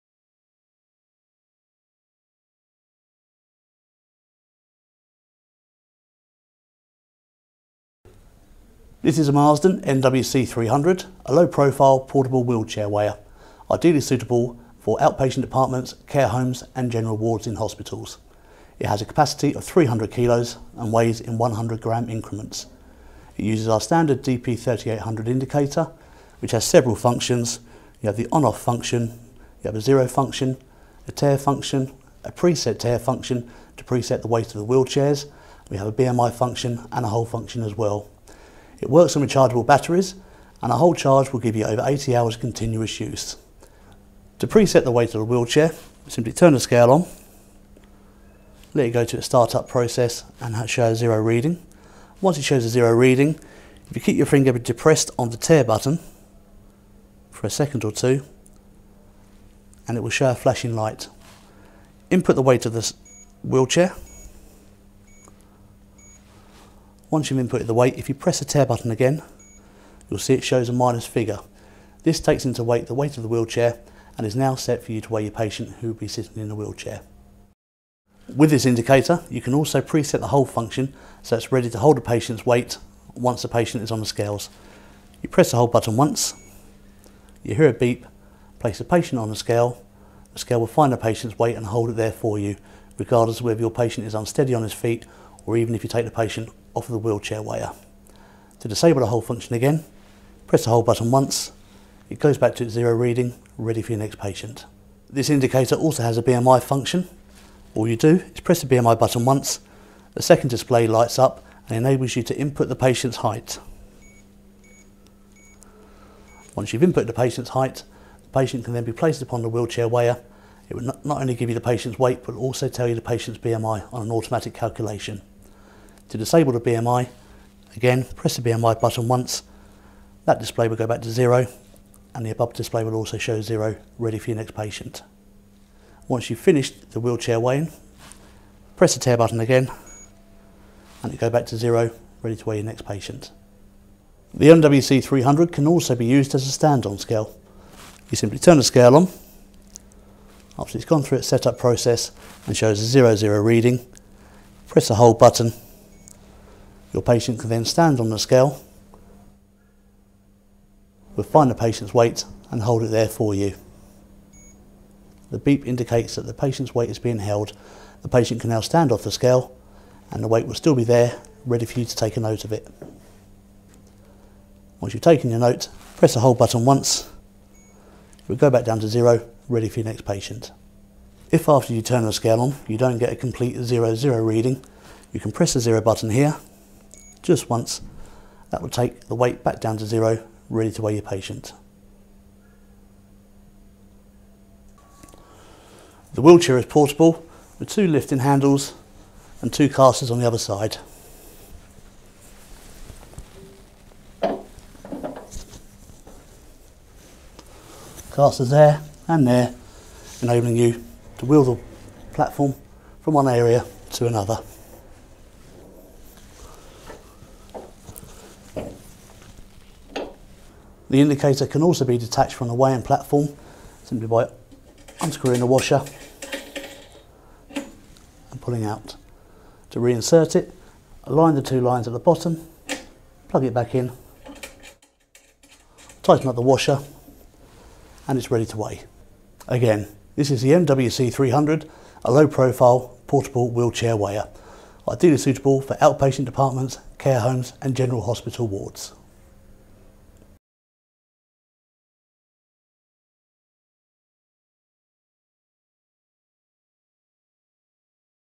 Marsden M-630, Portable Wheelchair Weigher
Product video for the Marsden M-630, portable wheelchair weigher.  Class III approved scale.